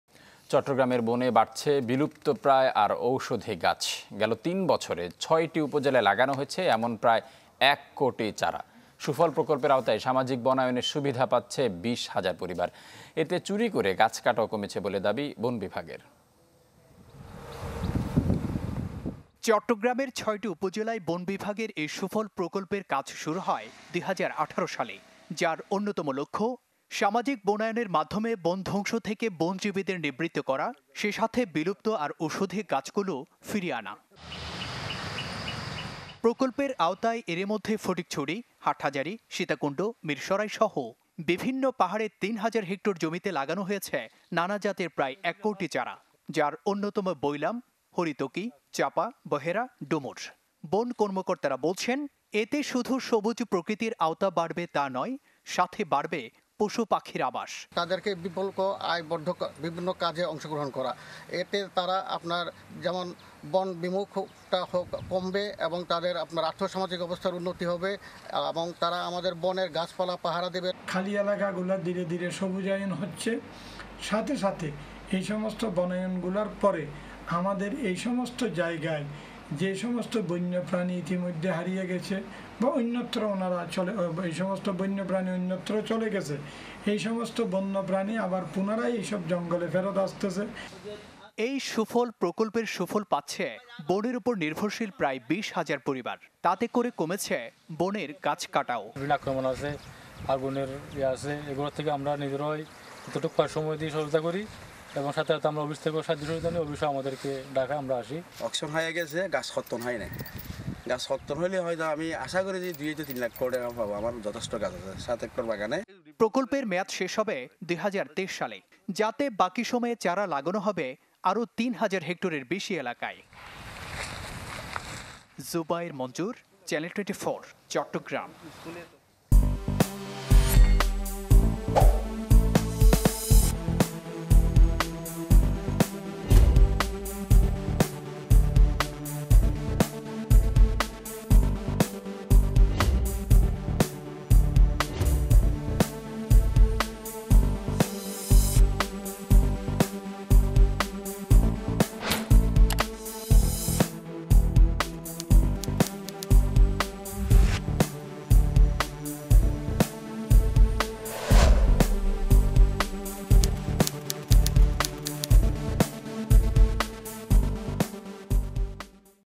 চট্টগ্রামে বাড়ছে বিলুপ্তপ্রায় ও ঔষধি গাছ , নতুন লাগানো হয়েছে ১ কোটি চারা
বনে বাড়ছে বিলুপ্তপ্রায় ও ঔষধি গাছ , নতুন লাগানো হয়েছে ১ কোটি চারা | Channel 24
Welcome to the Official YouTube Channel of Channel 24

»» Office Address:
Bangladesh.